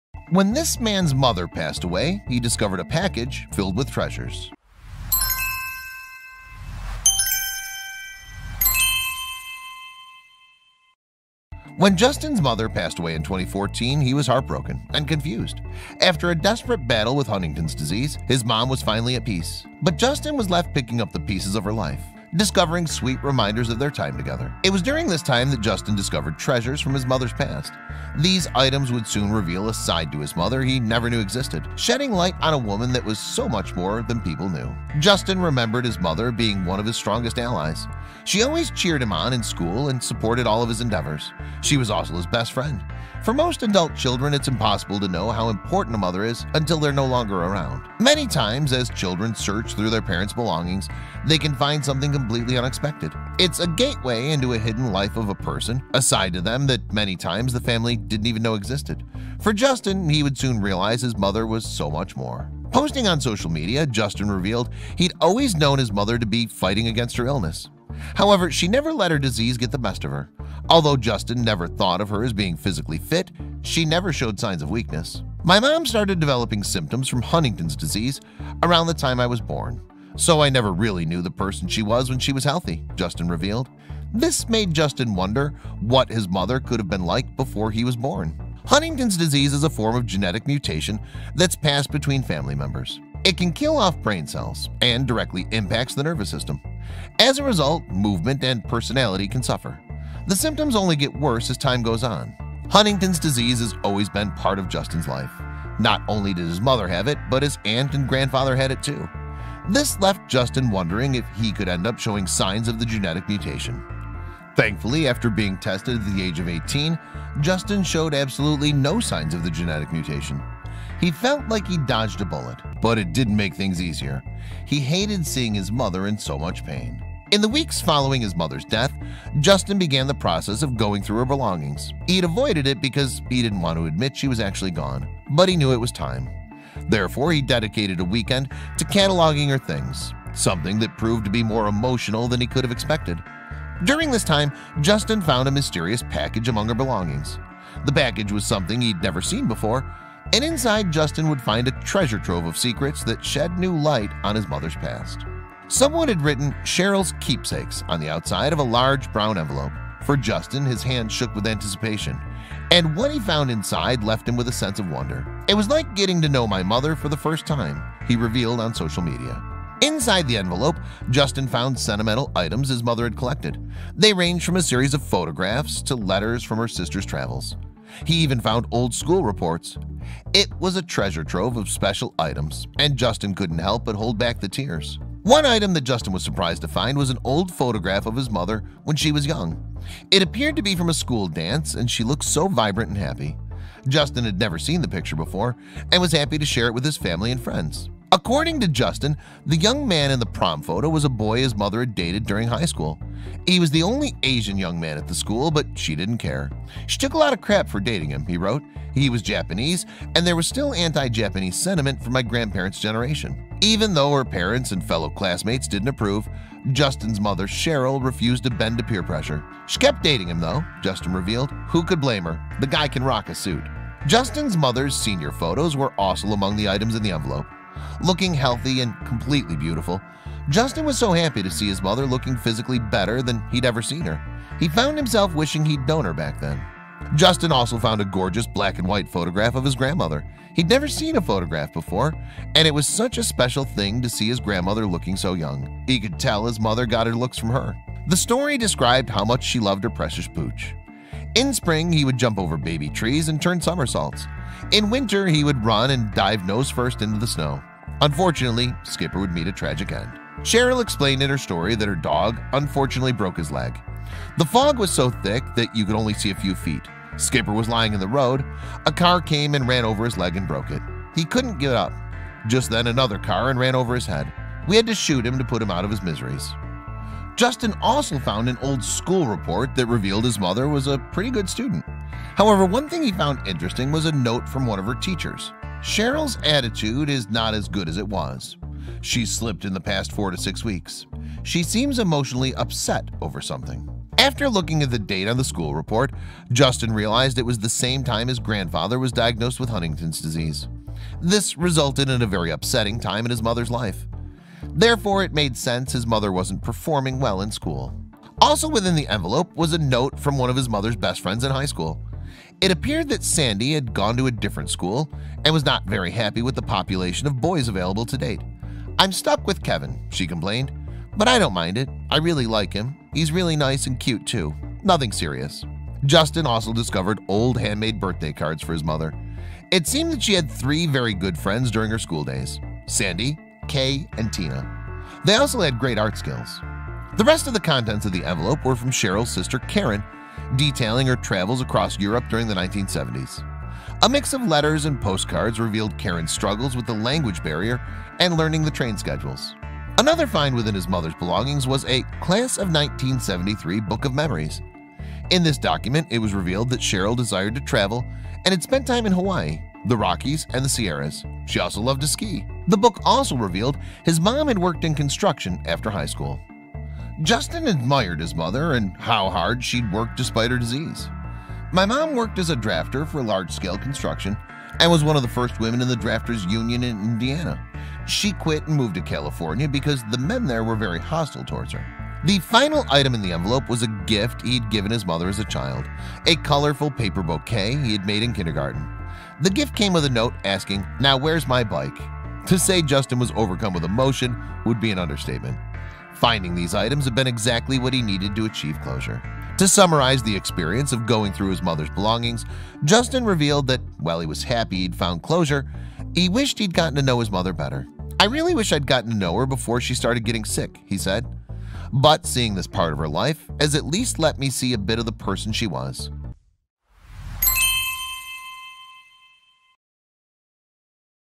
When This Man’s Mother Passed Away, He Discovered A Package Filled with Treasures
shortsyoutube When This Man’s Mother Passed Away, He Discovered A Package Filled with Treasures